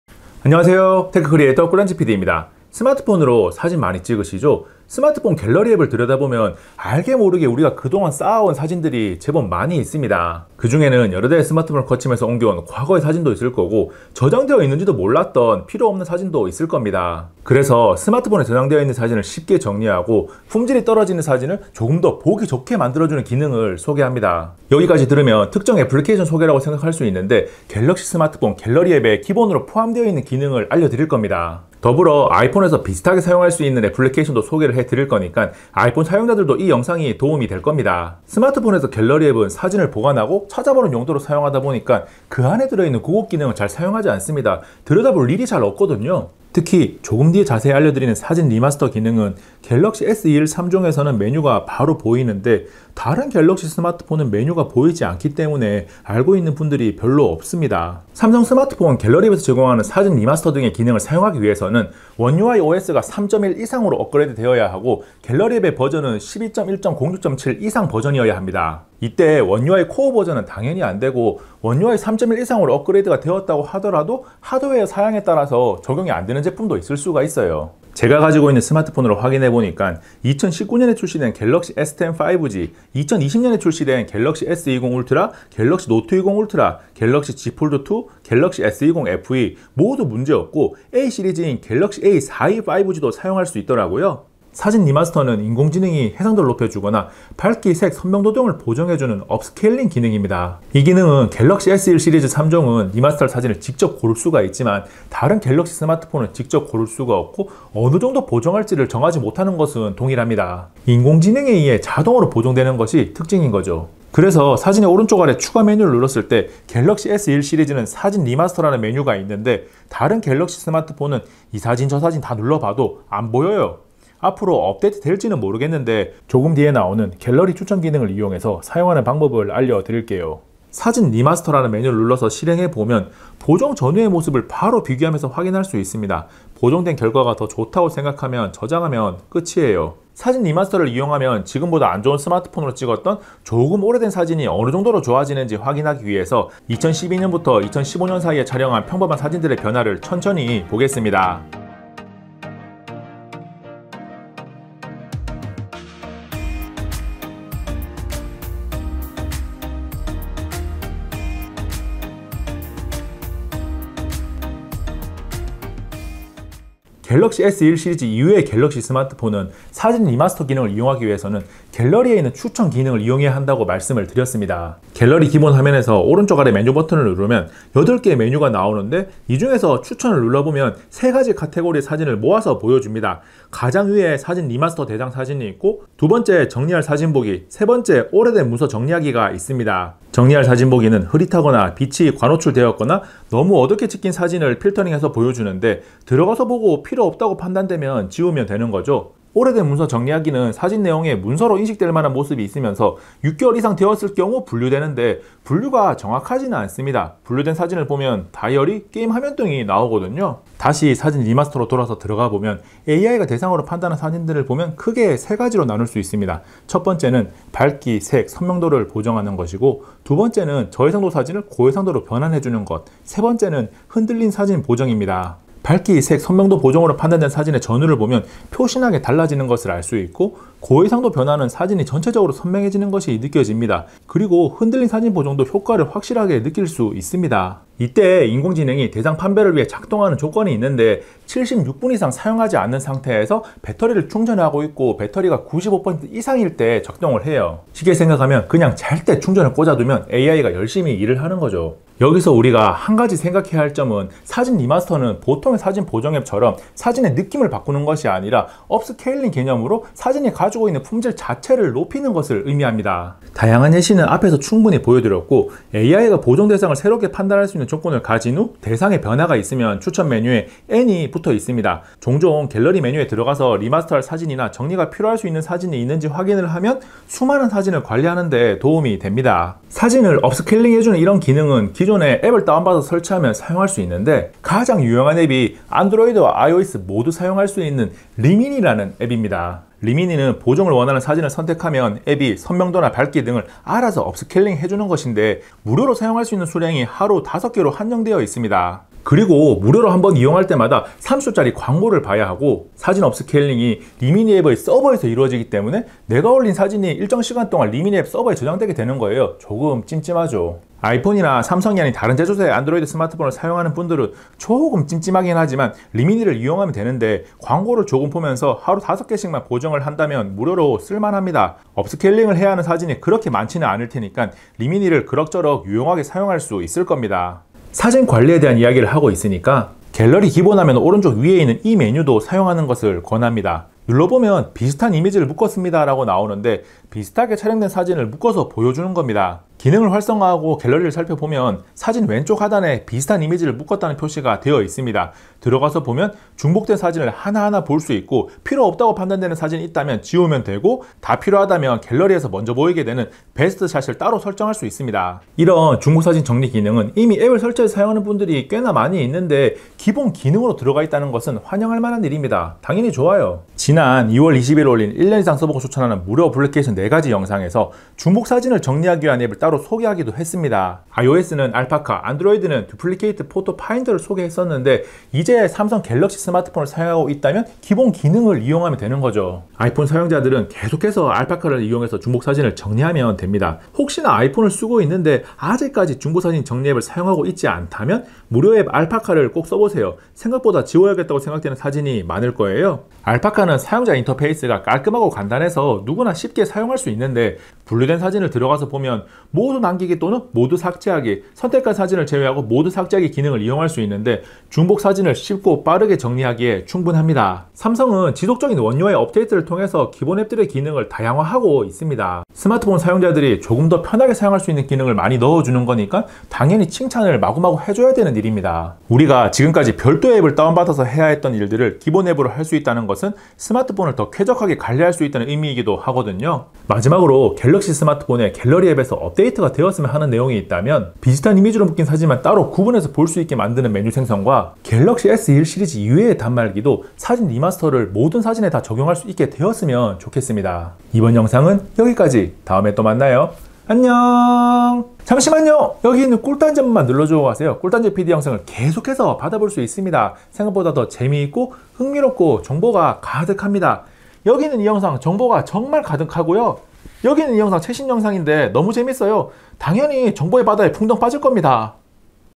갤럭시폰 사용자 1%만 아는 꿀 기능, 사진 리마스터 & 중복 사진 묶음! (아이폰은 Remini & ALPACA)
삼성 갤럭시 스마트폰 갤러리앱에는 자세히 살펴보지 않으면 알기 어려운 고급기능이 포함되어 있습니다. 사진 리마스터, 중복 사진 묶어서 보기 등의 기능이 포함되어 있는데, 어떻게 사용하고 사용할만한 기능인지 알려드립니다. 더불어 아이폰에서 비슷하게 사용할 수 있는 애플리케이션 Remini와 ALPACA도 소개합니다.

☞ 타임 스탬프
0:00​​​​ 스마트폰 사진 관리
0:58 사진 리마스터 조건
1:34 사진 리마스터 뭘까
2:11 사진 리마스터 성능
2:48 갤러리 추천 기능
3:30 사진 리마스터 3가지
3:57 인공지능 작동 조건
4:40 아이폰 업스케일링 Remini
5:27 중복 사진 정리
6:19 아이폰 중복사진 정리 ALPACA
7:07 갤러리앱 업데이트 바람
7:25 꿀PD 한 마디

구독을 누르면 꿀단지PD 영상으로 빠르게 만나볼 수 있어요!